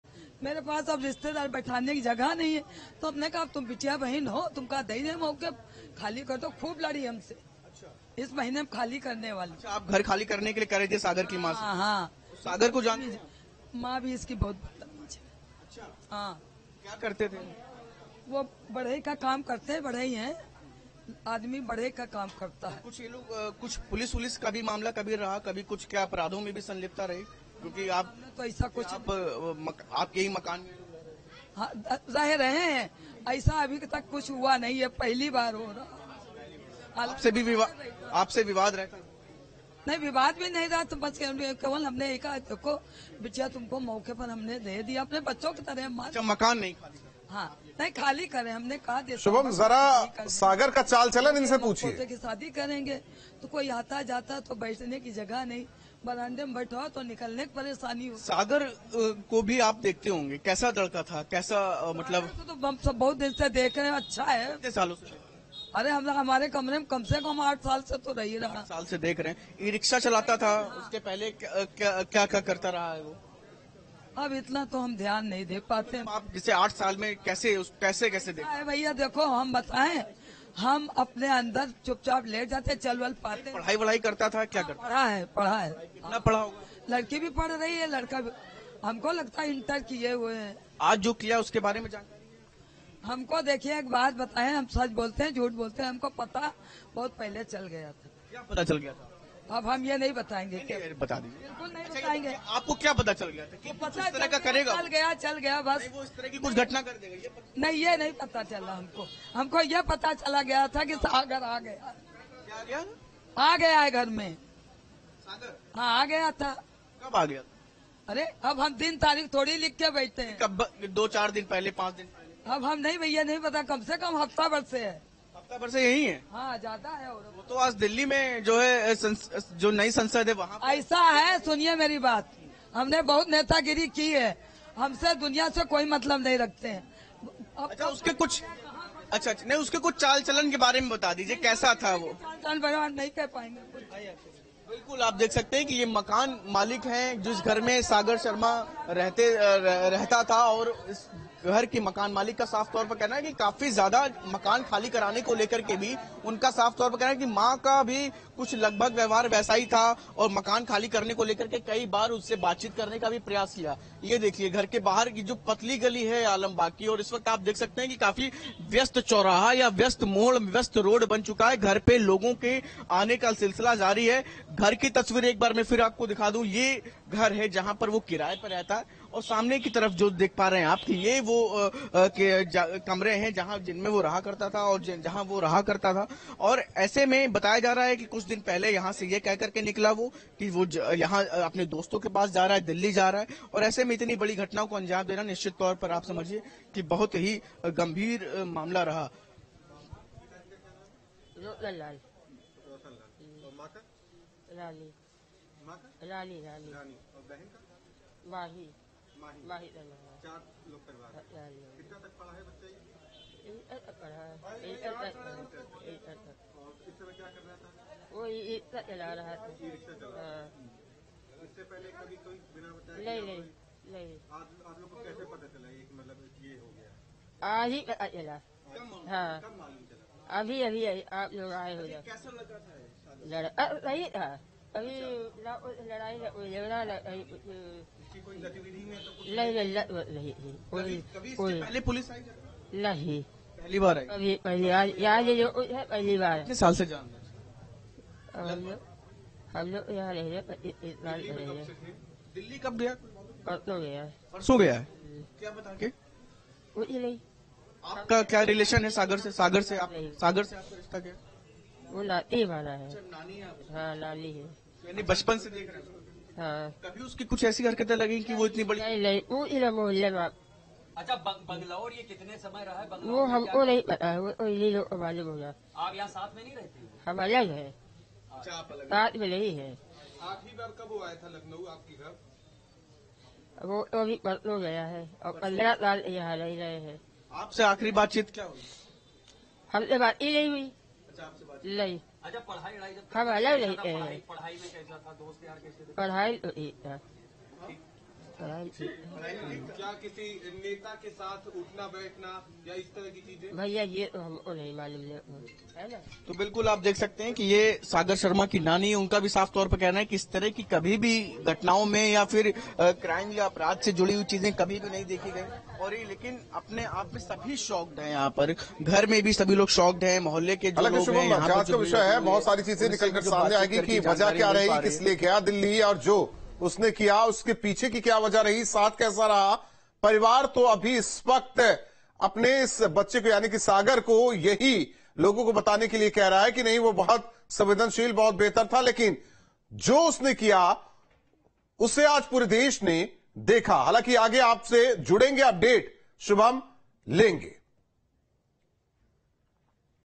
Parliament Security Breach: आरोपी Sagar Sharma के पड़ोसियों ने किया चौंकाने वाला खुलासा!
News State Apps डाउनलोड करें:
Download Android and iOS Apps for more updates: